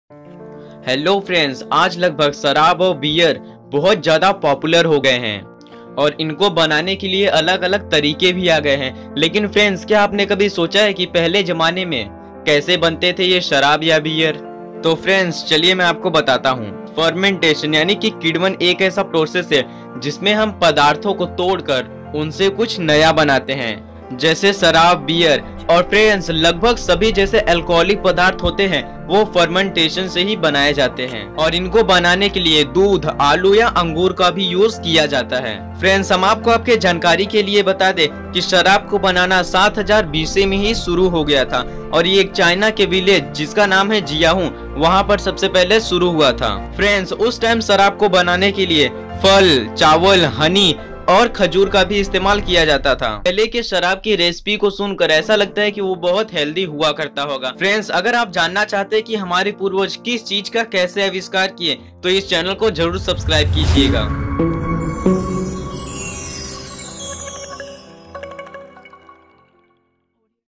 सबसे पहले शराब किसने बनाया।। शराब क्यों बना था जानकर हैरान रह जाएंगे
hello friends in this video I have talked about how was the wine produced it was produced due to the fermentation so hope you like this video please like share comment and subscribe to our channel